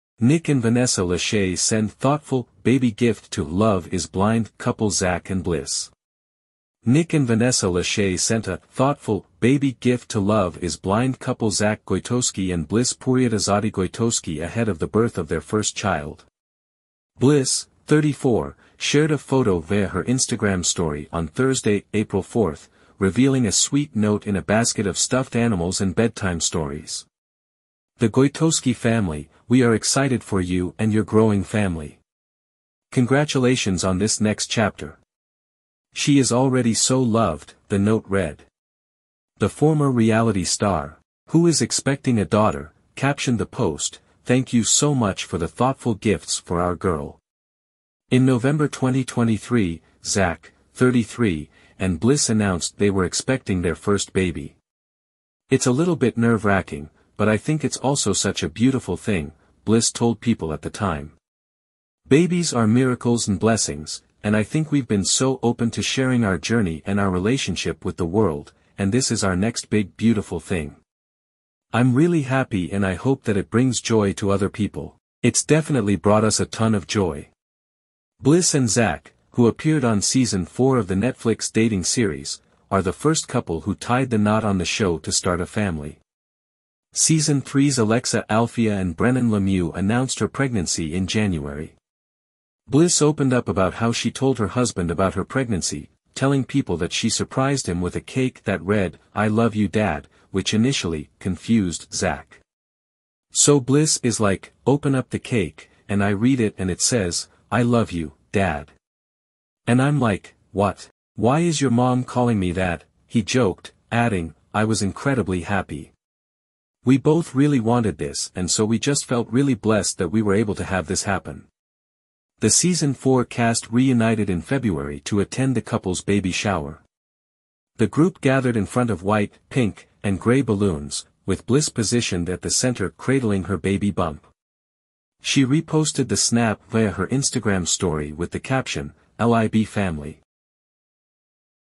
Nick and Vanessa Lachey Send ‘Thoughtful’ Baby Gift to ‘Love Is Blind’ Couple Zack and Bliss.
In a heartwarming gesture, Nick and Vanessa Lachey have extended their love and congratulations to 'Love Is Blind' stars Zack and Bliss ahead of their impending parenthood journey.